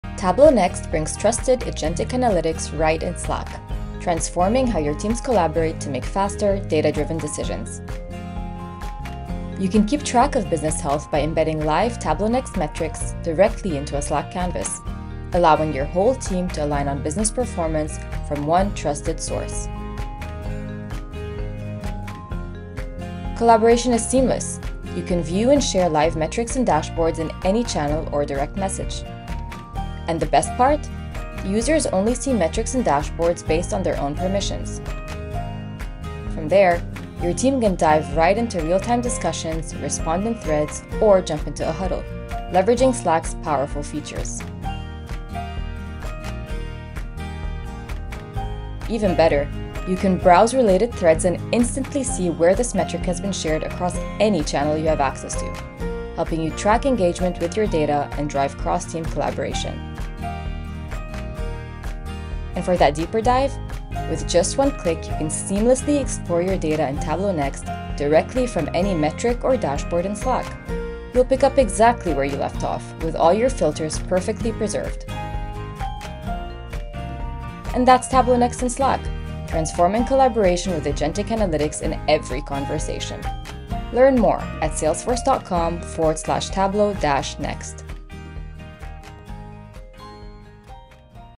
Tableau Next and Slack Demo
Tableau Next infuses agentic analytics directly into your team's Slack workflow. Share permission-aware live metrics and dashboards in channels, direct messages, and canvases, ask agents natural language questions for accurate insights, and collaborate on a single source of truth—all from Slack, with seamless click back to Tableau Next for deeper data exploration.